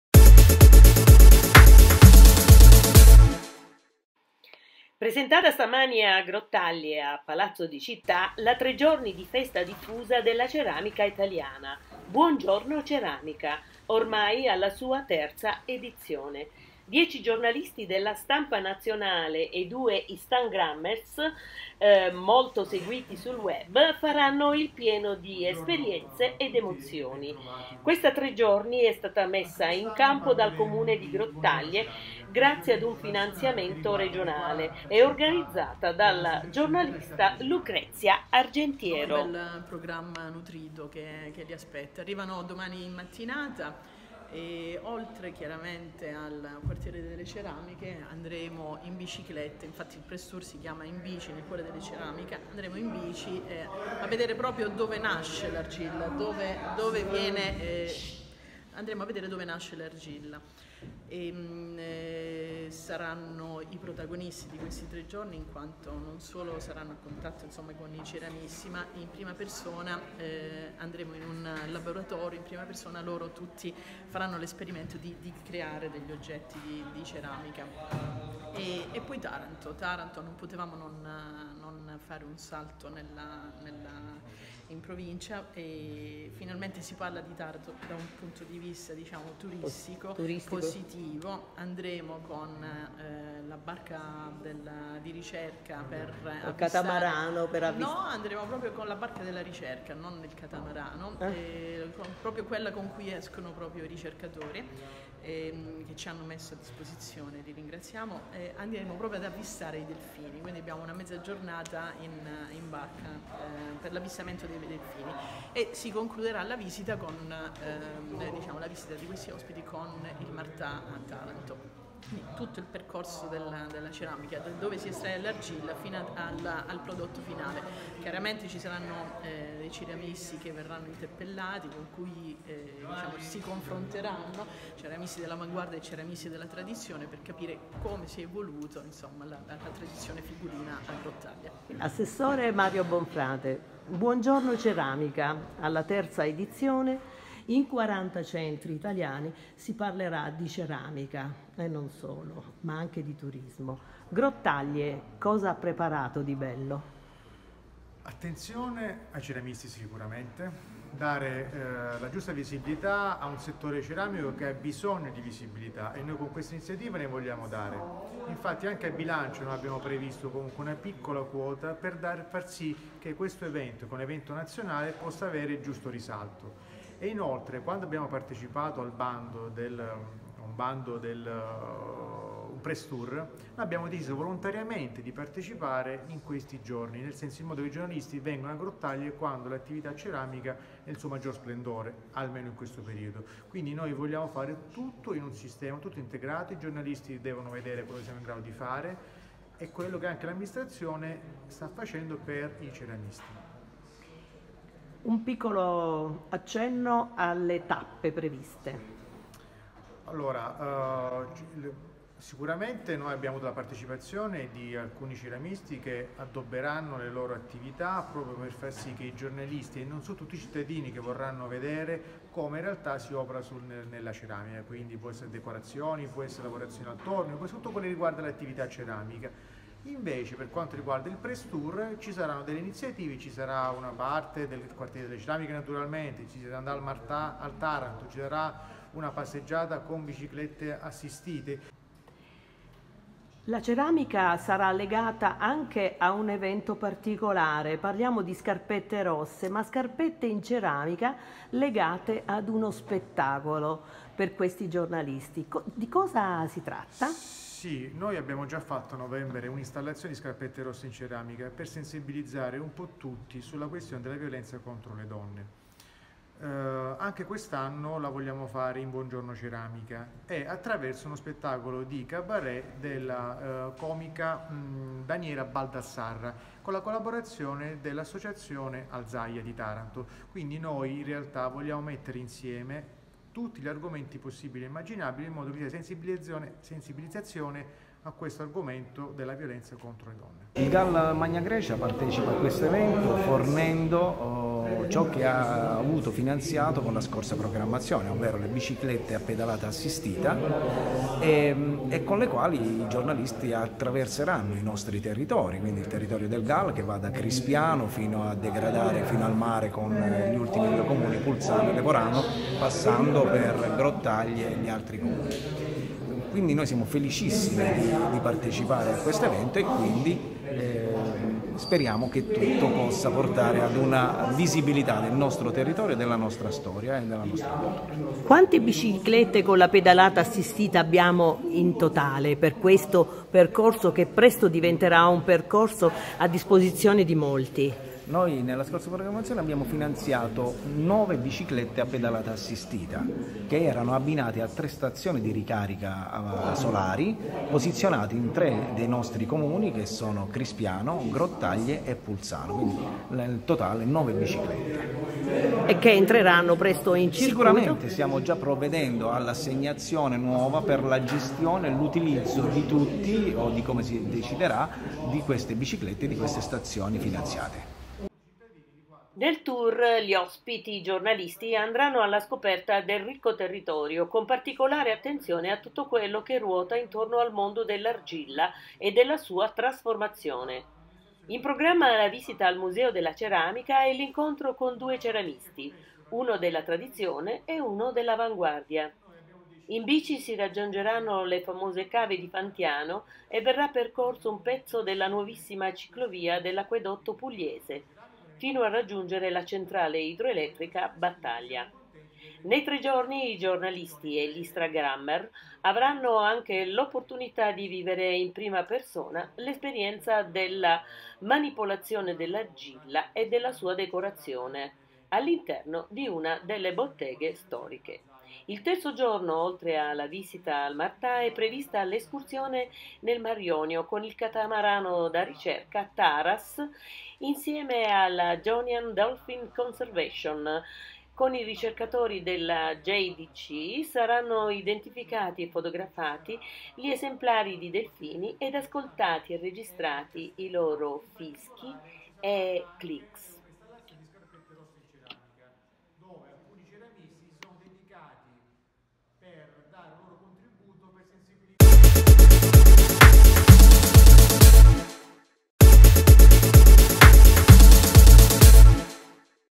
"Buongiorno Ceramica" press tour a Grottaglie e dintorni
“In bici nel cuore della ceramica”
Educational per giornalisti e instragrammers specializzati in turismo

Faranno il pieno di esperienze e di emozioni i 10 giornalisti della stampa nazionale (Viaggi di Repubblica, Dove del Corriere della Sera, Il Messaggero, La Stampa, Vanity Fair, Donna Moderna, Elle, TouringClub e 2 Instagrammers molto seguiti sul web”), che saranno ospiti a Grottaglie a partire da venerdi 17 maggio, in occasione di “BUONGIORNO CERAMICA”, evento nazionale organizzato dall’AICC (Associazione Italiana Città della Ceramica) .

Tre i giorni messi in campo dal Comune di Grottaglie sostenuto dalla Regione Puglia, grazie ad un finanziamento ottenuto con l’Avviso 6.8 e organizzati dalla giornalista Lucrezia Argentiero, per un viaggio che parte dalle gravine della Murgia e arriva allo Ionio.
Nel tour gli ospiti andranno alla scoperta del ricco territorio, con particolare attenzione a tutto quello che ruota intorno al mondo dell’argilla e della sua trasformazione. In programma la visita al Museo della Ceramica e l’incontro con due ceramisti, uno della tradizione e uno dell’avanguardia. In bici si raggiungeranno le famose Cave di Fantiano e verrà percorso un pezzo della nuovissima Ciclovia dell’Acquedotto Pugliese fino a raggiungere la Centrale Idroelettrica Battaglia.
Nei 3 giorni i giornalisti e gli instagrammers avranno anche l’opportunità di vivere in prima persona  l’esperienza della manipolazione dell’argilla e decorazione , all’interno di una delle botteghe storiche. Il terzo giorno, oltre alla visita al MARTA è prevista l’escursione nel Mar Jonio con il catamarano da ricerca TARAS insieme alla Jonian Dolphin Conservation. Con i ricercatori della JDC saranno identificati e fotografati gli esemplari di delfini ed ascoltati e registrati i loro fischi e clicks.

“Quando si parla di valorizzazione del territorio con l´intento di avviare l´industria del turismo - ci ha tenuto a sottolineare Ciro D’Alò, sindaco del Comune di Grottaglie– significa, puntare al turismo esperienziale e far vivere al viaggiatore le stesse esperienze di un abitante del posto. Ai giornalisti ospiti offriremo, infatti, la possibilità di vivere esperienze autentiche, che arricchiranno il loro bagaglio di conoscenze, grazie alle quali si creerà un legame intimo e profondo con il territorio, un mix di emozioni che saprà calamitare l'attenzione di nuovi viaggiatori.

 “L’eductour per giornalisti è una delle più efficienti forme di veicolazione del prodotto turistico - sottolinea Mario Bonfrate, assessore al turismo del Comune di Grottaglie - è di fondamentale importanza il diretto contatto tra operatori economici (hotel, ristoranti) e media, allo scopo di ottenere, visto l’alto livello professionale, segnalazioni puntuali sulla stampa”.
Ora non ci resta che attendere di vedere le emozioni riportate sulla carta e sul web!